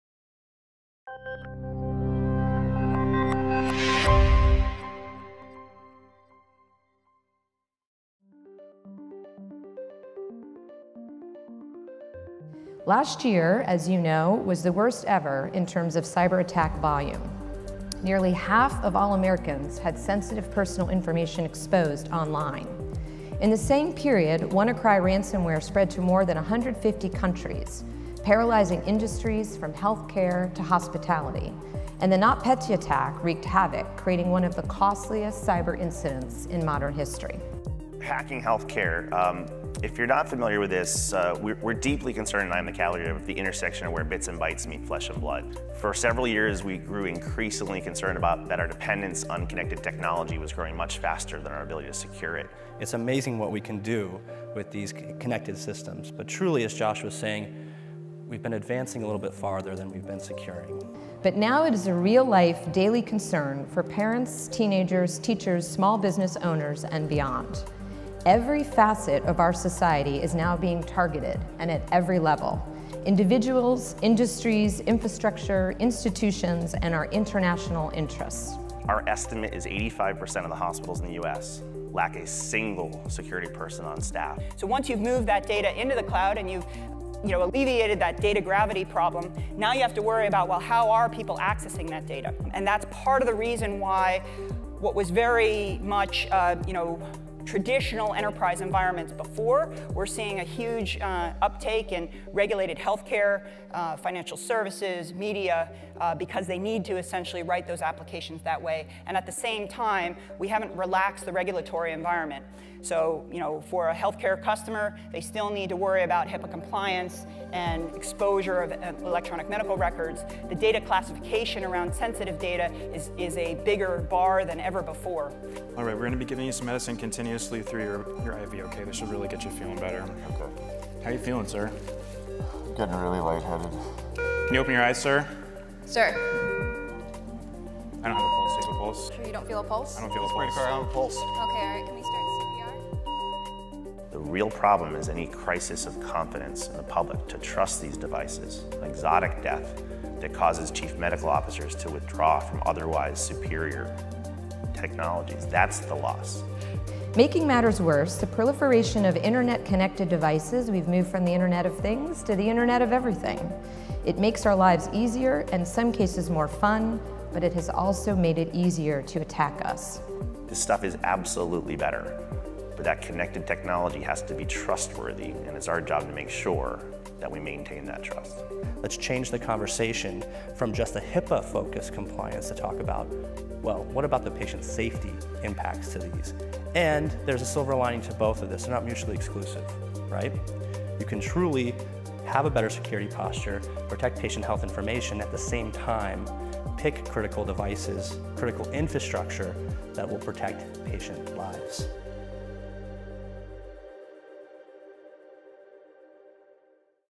Healthcare and Cybersecurity | Highlights from RSAC 2018
Cybersecurity experts say healthcare is the next frontier of hacking and cyber-attacks. That’s just one of the many reasons why protecting our healthcare system was a widely discussed topic at RSA Conference 2018. From modernizing authorization and data access of electronic health records, to hacking connected medical devices, this video provides highlights of security and healthcare discussions at RSAC 2018.